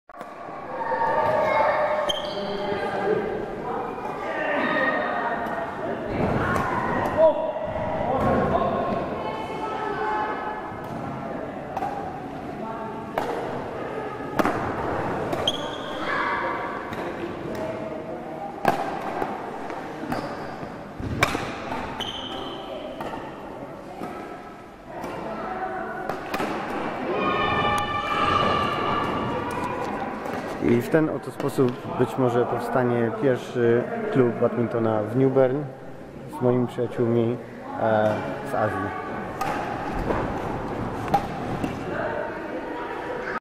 Badminton w UK
Wspólnie z przyjaciółmi z Indii i Malezji będziemy próbować zbudować własny klub, by móc w przyszłości grać w amatorskiej lidze w Newcastle w Wielkiej Brytanii. Obecnie poszukujemy graczy, najlepiej takich, którzy już mieli jakieś doświadczenie na korcie.